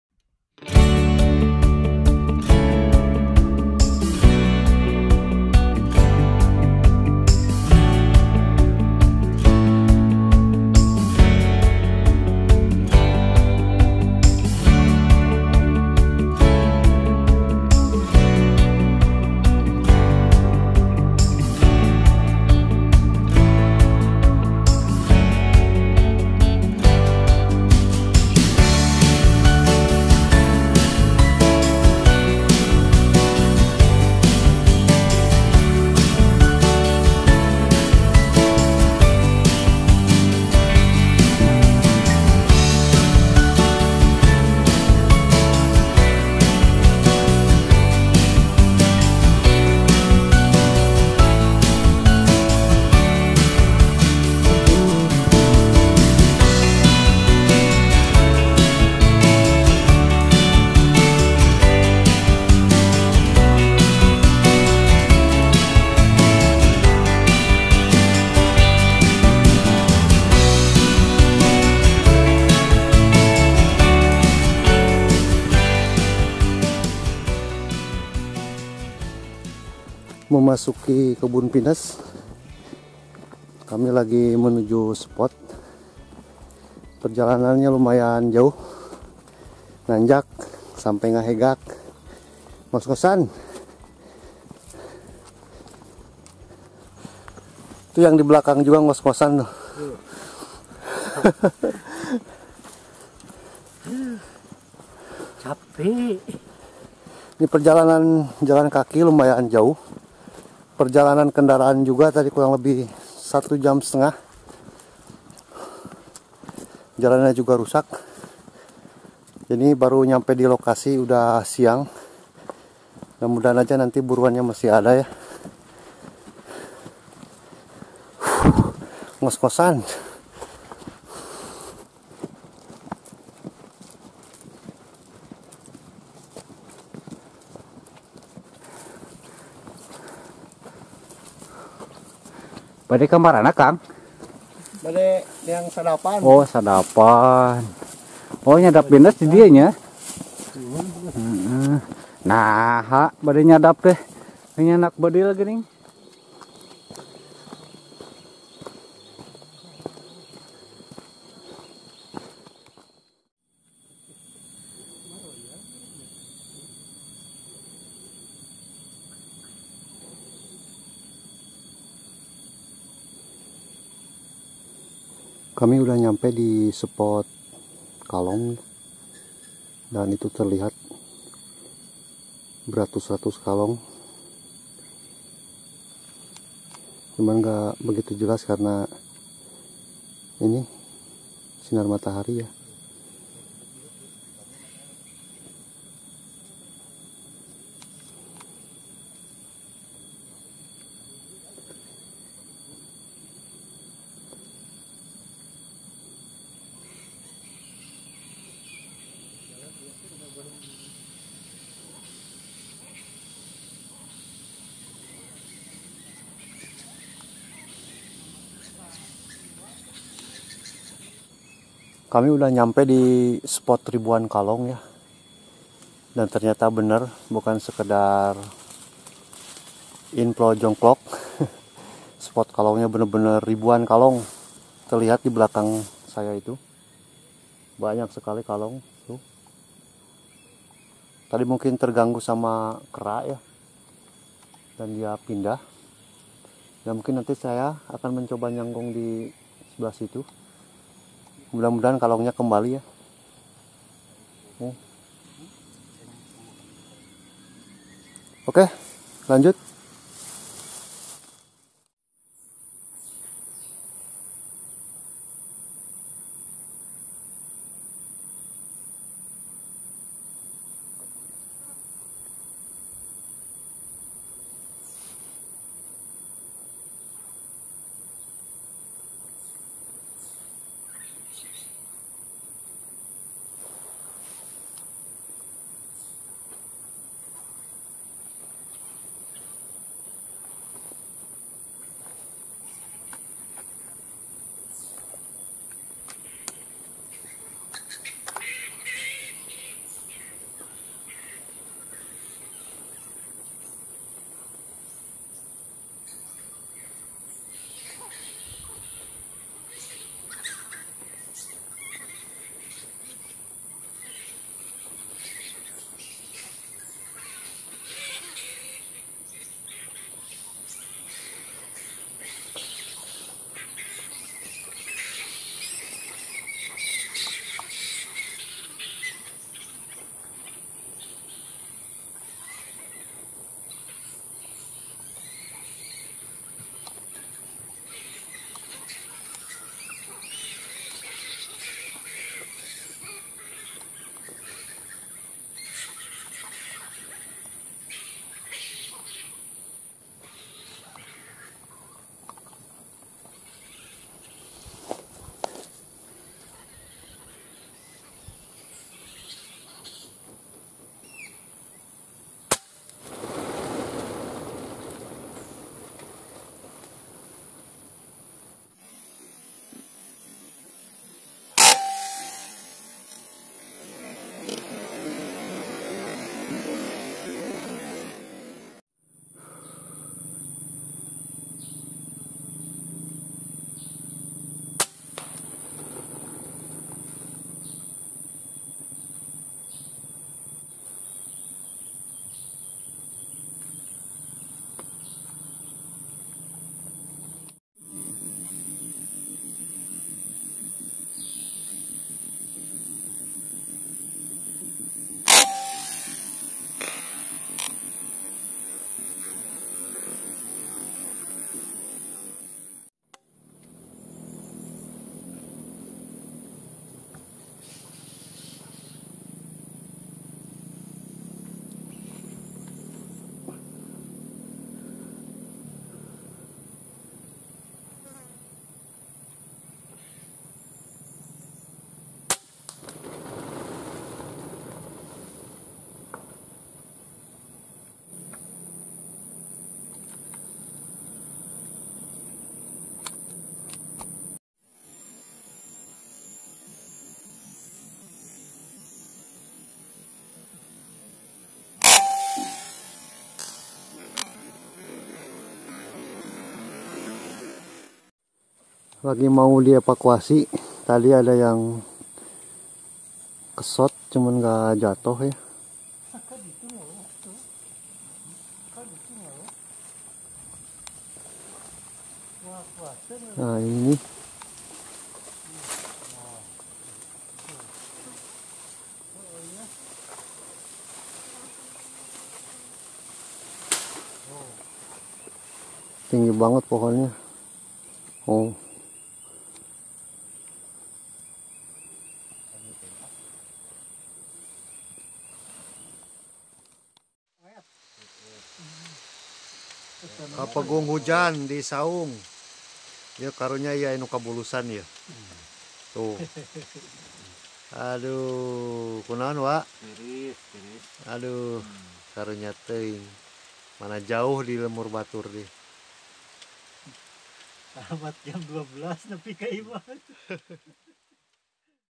Spot Langka & Jarang Ditemui || Berburu Kalong Di Markasnya
kali ini kami berburu di spot ribuan kalong, spot yang langka dan jarang ditemui. Tantangan tersendiri berburu kalong kali ini, yakni spot yang sangat jauh dan kalongnya pun kebal,dalam arti sekali shot tidak langsung jatuh. Semua point yang di dapat juga tidak langsung jatuh tapi nyangkut di ranting pohon pinus yang menjulang tinggi, sangat sulit untuk mengevakuasinya. Bagaimana keseruannya, simak aja videonya sampai selesai.

TERIMA KASIH 🙏🙏